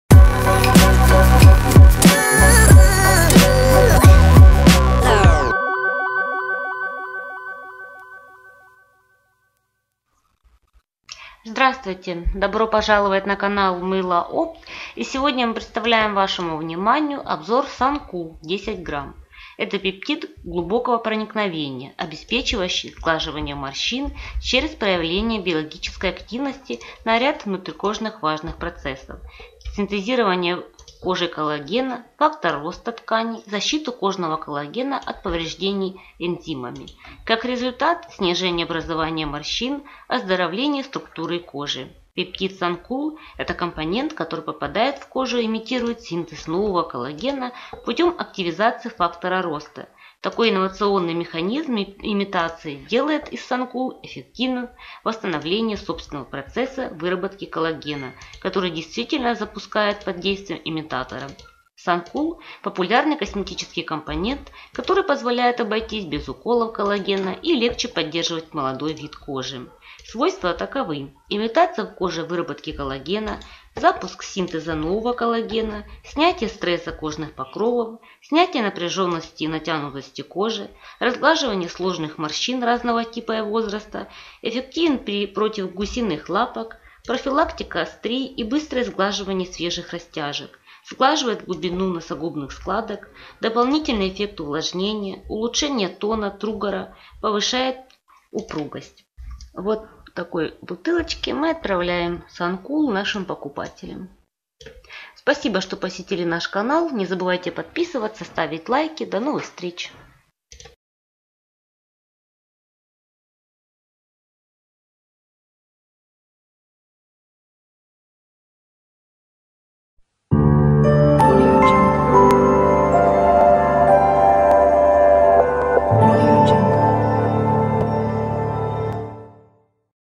Syn-Coll (Сан Кул) - пептид глубокого проникновения // Пептидная косметика
Видео обзор Syn-Coll (Сан Кул) - пептид глубокого проникновения от магазина Мыло-опт